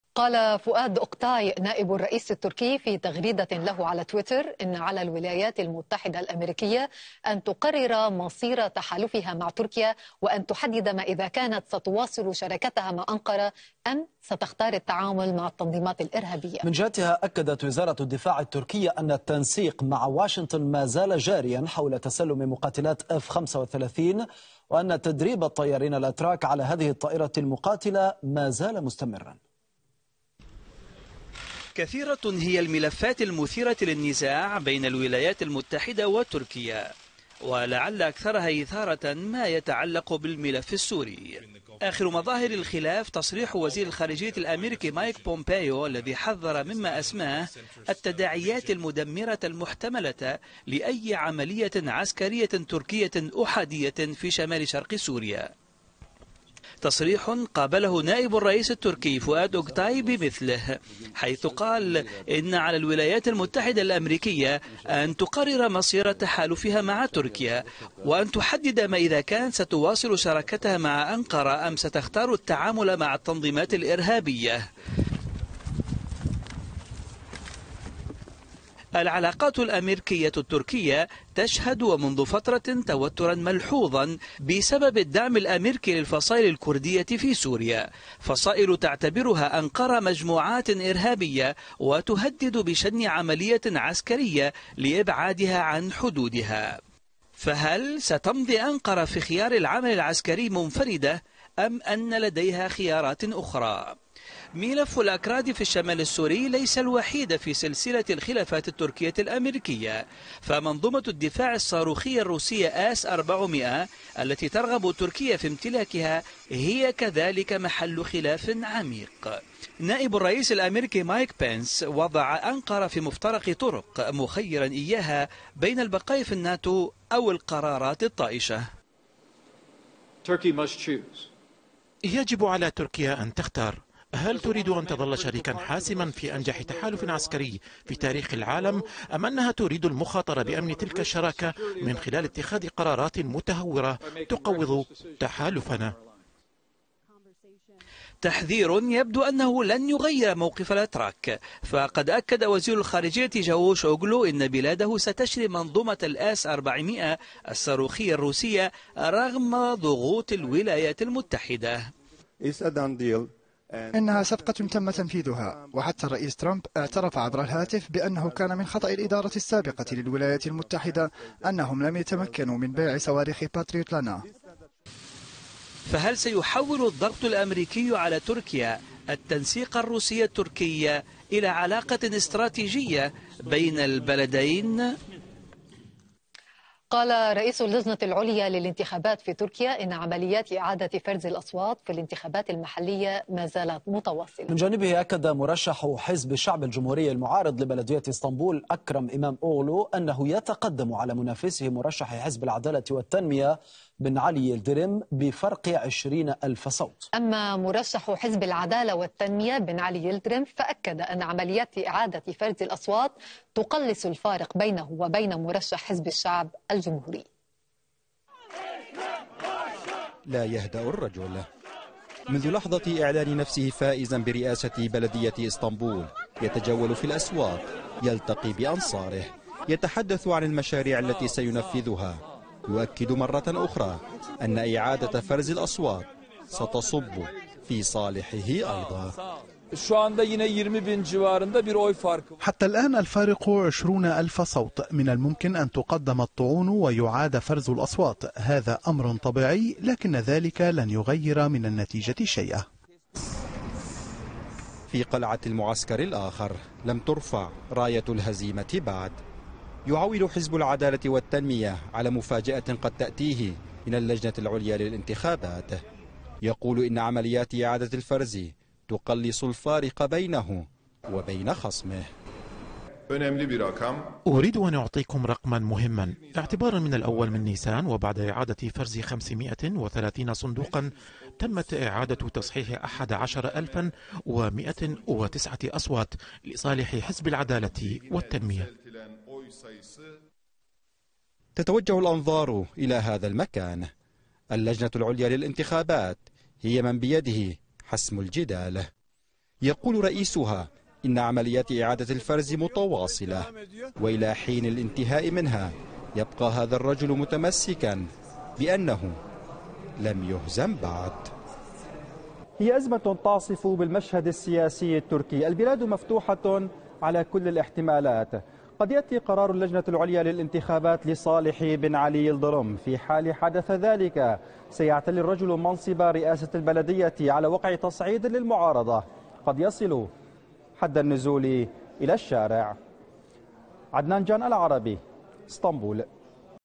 العربي اليوم | العلاقات التركية الأميركية .. تصاعد الخلافات بين البلدين
قال فؤاد أقطاي نائب الرئيس التركي في تغريدة له على موقع التواصل الاجتماعي تويتر، إن على الولايات المتجدة الأميركية أن تقرر مصير تحالفها مع تركيا وأن تحدد إذا ما كانت ستواصل شراكتها مع أنقرة أم ستختار التعامل مع التنظيمات الإرهابية.